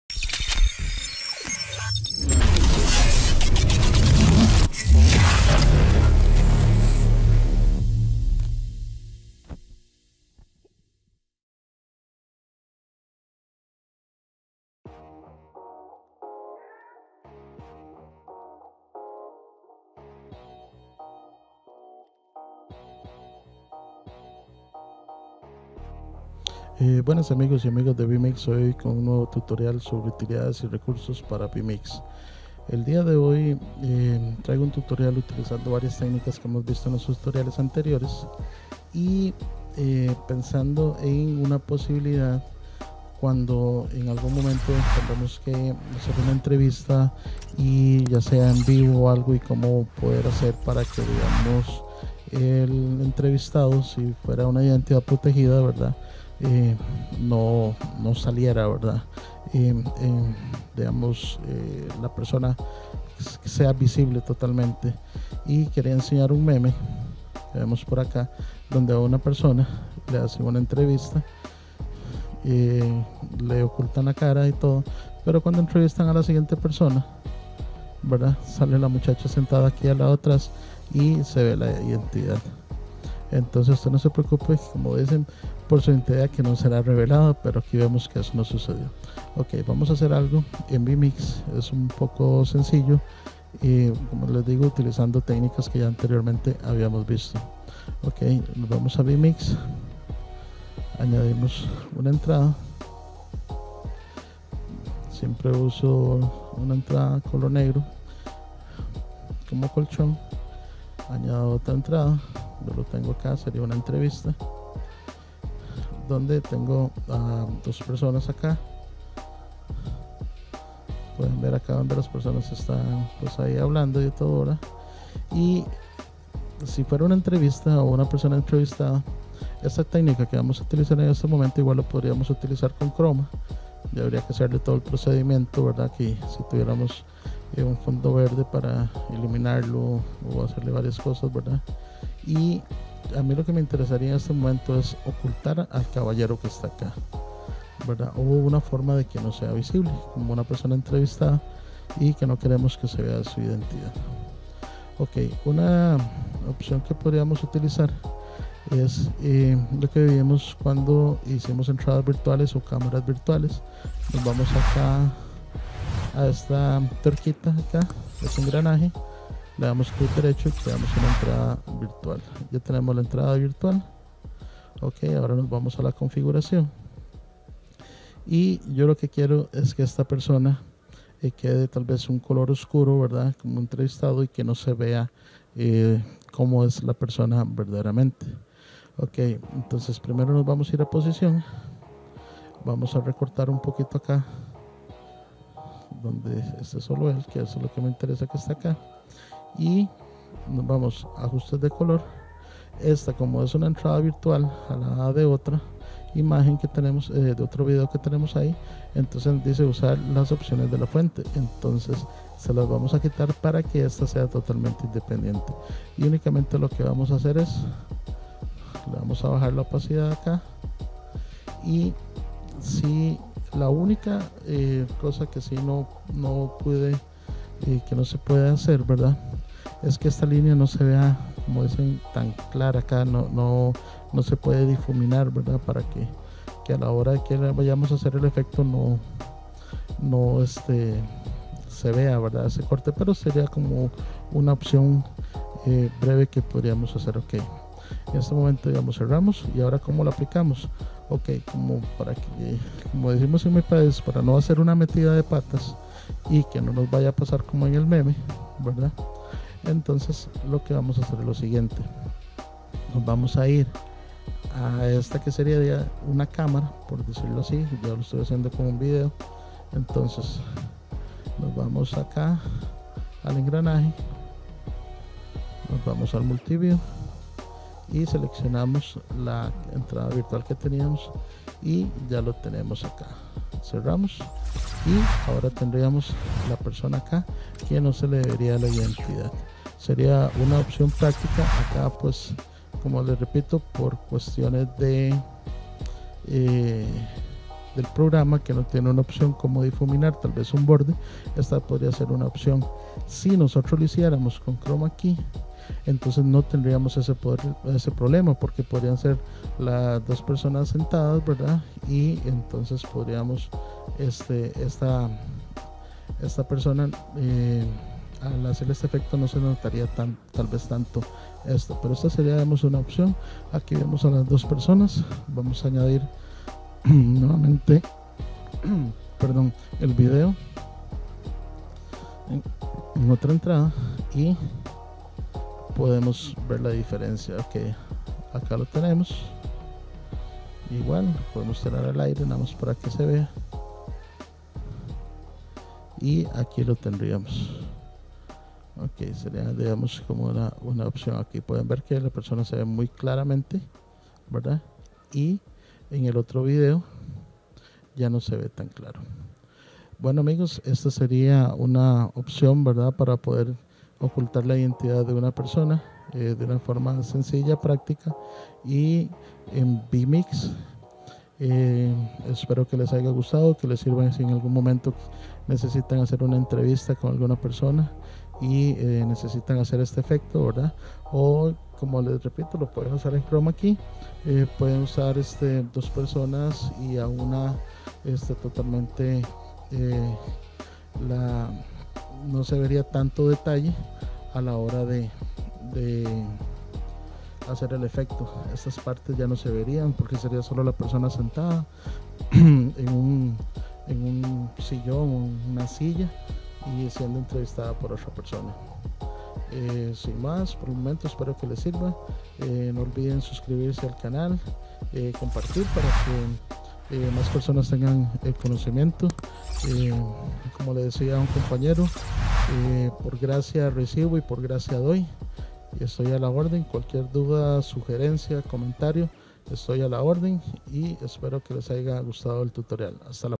Efecto Identidad Protegida para entrevistas en Vmix
Acá les muestro una opción de como proteger la identidad de una persona que esta siendo entrevistada y queremos que esa persona este en el anonimato usando Vmix